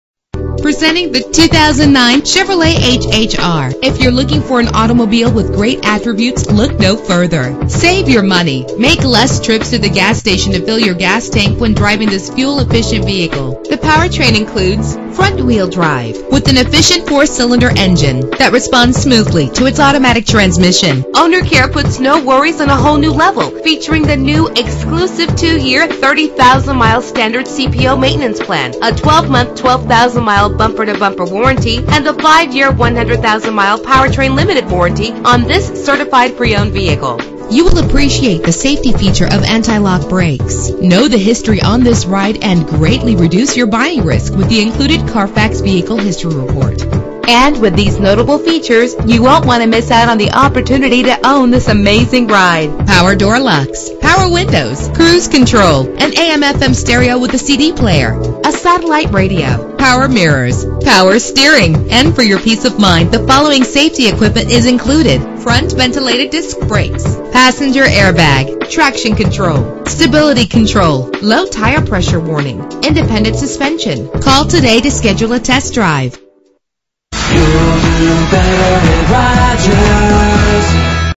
2009 Chevrolet HHR - Shelby NC
Year: 2009
Make: Chevrolet
Model: HHR
Trim: LS
Engine: 2.2 liter inline 4 cylinder 16 valve
Transmission: Automatic
Color: Red
Mileage: 22670
Address:
4423 E. Dixon Blvd